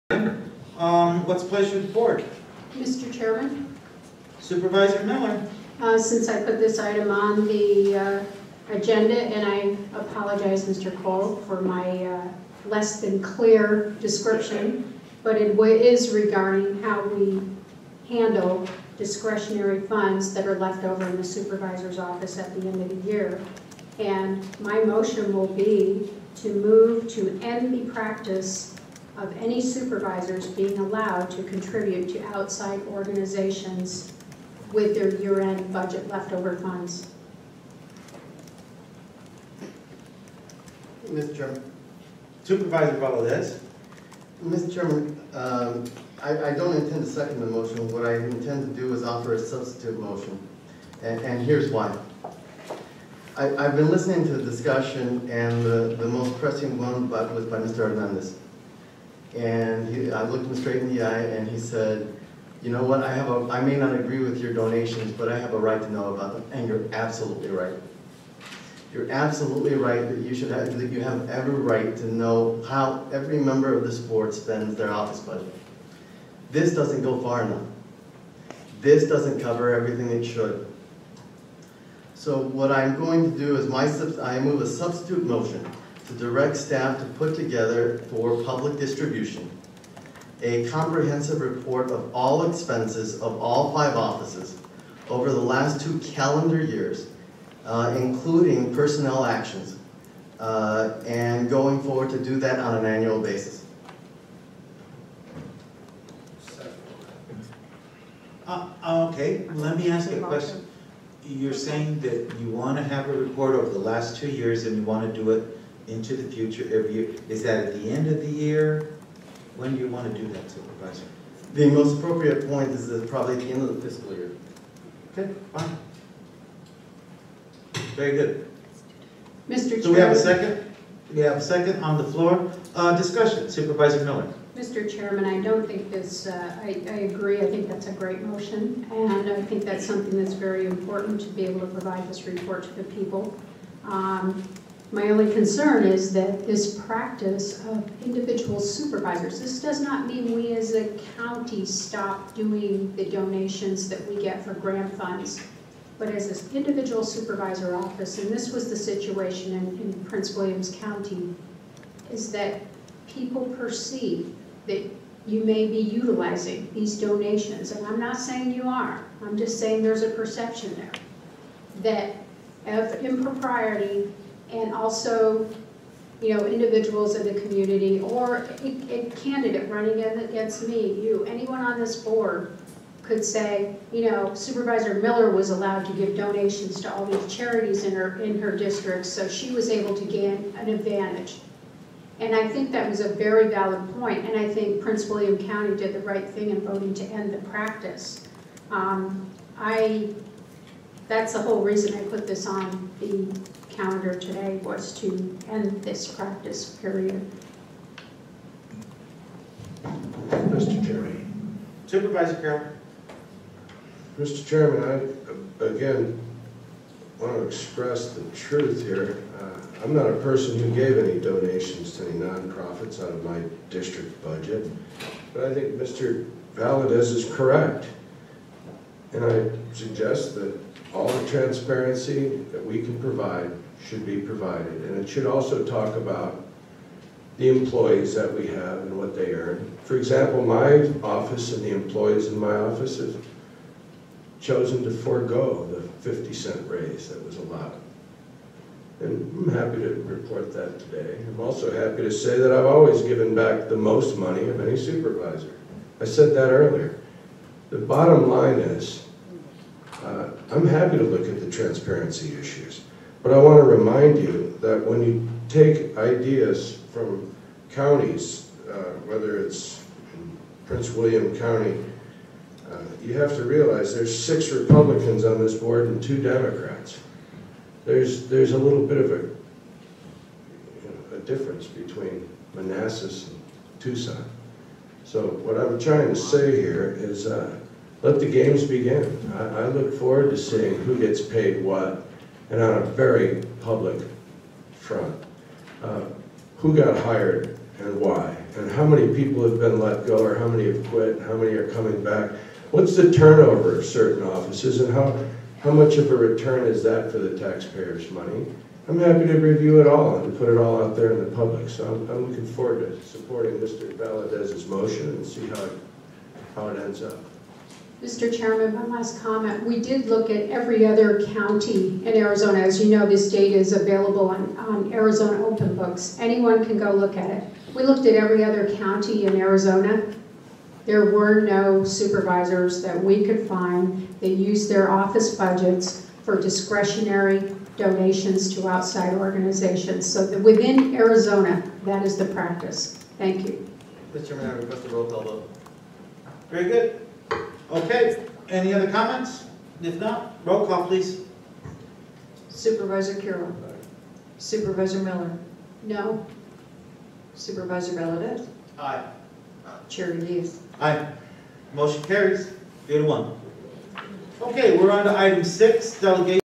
Discretionary Spending September 16, 2014 -Vote to End the Practice
Supervisor Miller was the only one to vote to put an end to the practice of Pima County Supervisors using office budget monies, taxpayer dollars, to make donations to outside organizations and charities of their choice.  Instead, the Board voted for a more transparent way of how they use their tax dollars.  Unfortunately, this 'more transparent'  practice is already available to the public to view by visiting openbooks.az.gov and Supervisors Carroll, Bronson, Elias & Valadez didn't want to stop their ability to spend taxpayer dollars in a questionable fashion.